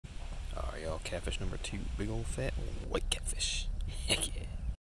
FAT CAT!
I was fishing and caught this fat catfish!